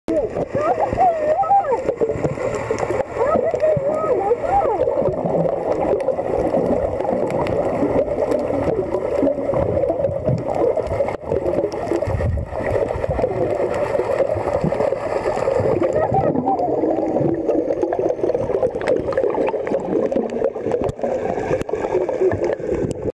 Green Moray Eel encounter at Hen & Chicken's Reef in Islamorada, Florida Keys
Green Moray Eel surrounded by variety of Coral Reefs and fish.